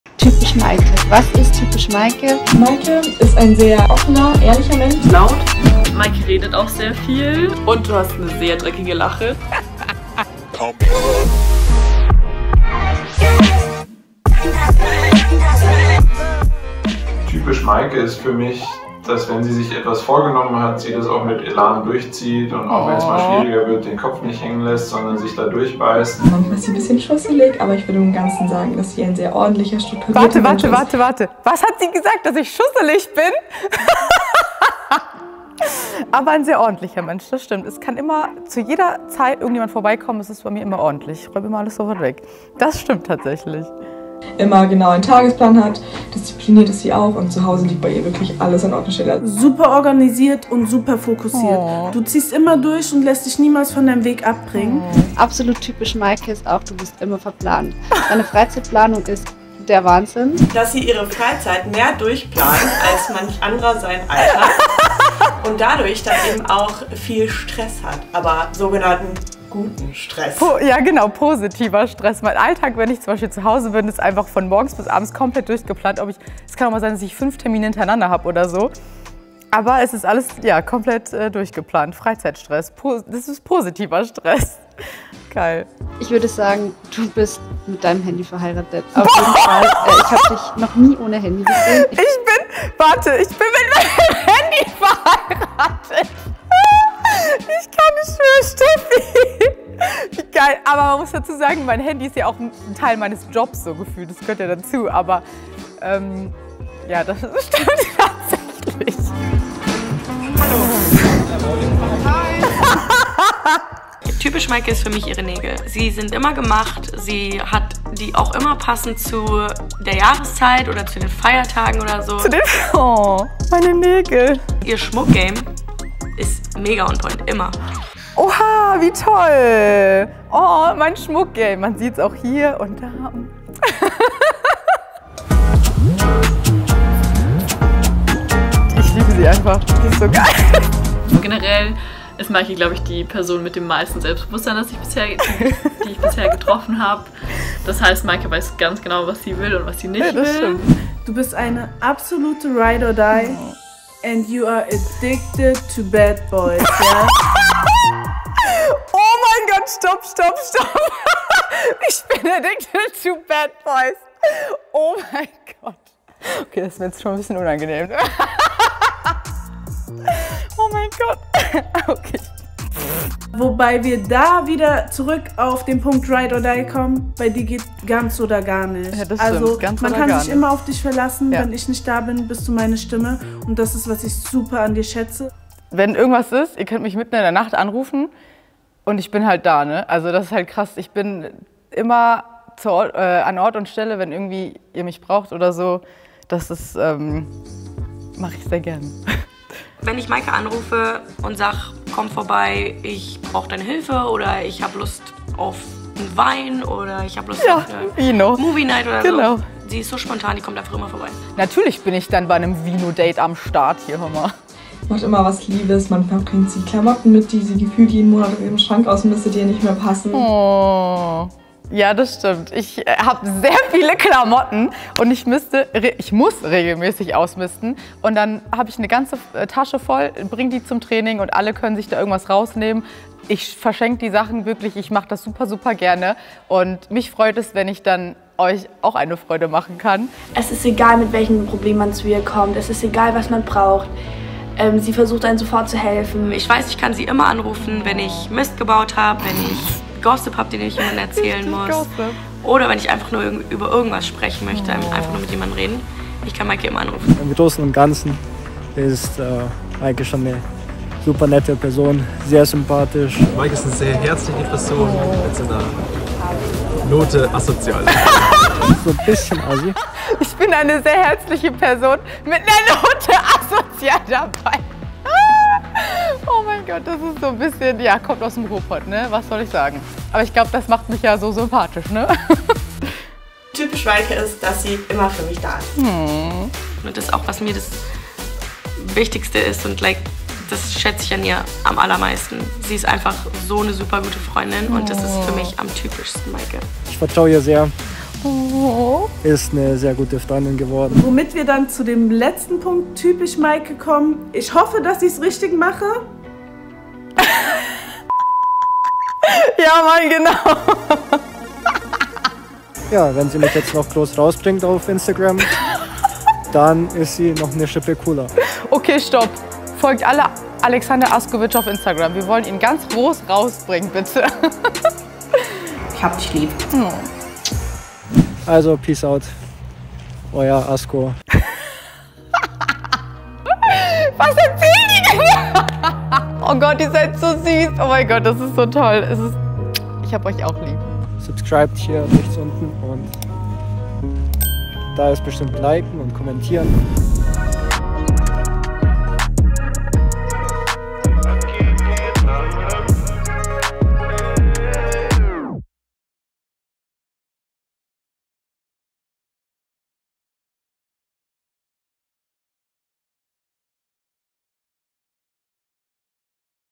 Was ist "Typisch Maike"? Part 2 😎
Hello und Herzlich Willkommen zu meinen neuen Video 📹

Weiter gehts es mit Part 2, wo meine Freunde und Trainingskollegen erzählen was typisch Maike ist.

Ich sag nur schusselig und Nägel 😂

Viel Spaß ✌🏻